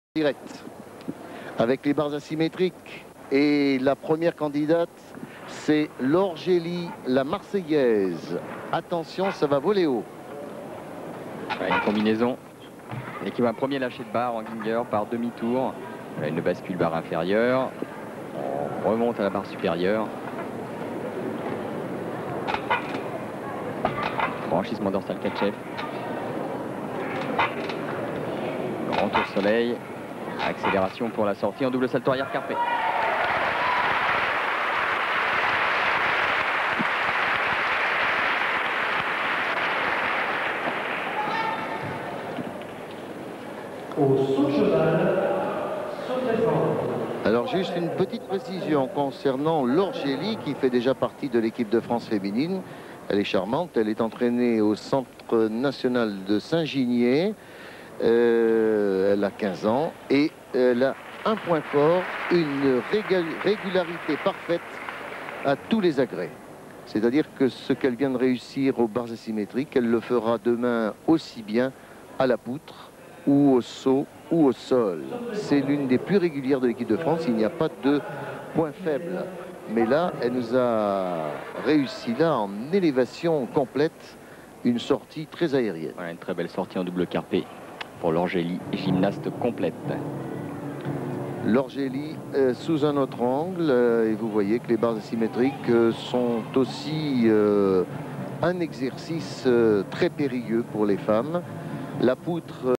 Laure GELY barres - 1994 Championnats de France AA
Laure GELY - Barres asymétriques

9.450
(6e par appareil)